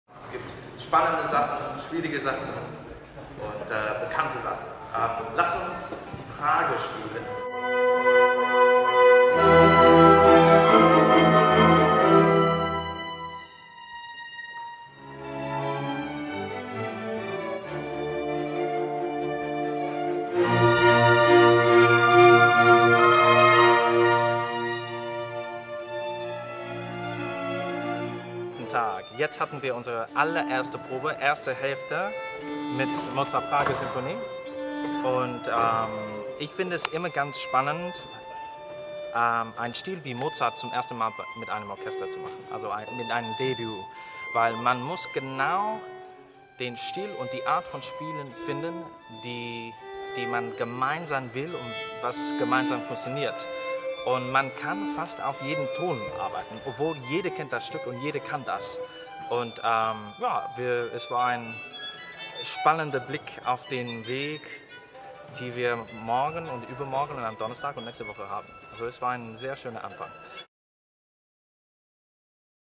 Probentag #2 in der Laeiszhalle mit Harry Ogg
Dirigent Harry Ogg berichtet vom ersten Probentag zum 1. VielHarmonie-Konzert am 27. Oktober um 19:30 Uhr in der Laeiszhalle. Mehr Infos und Tickets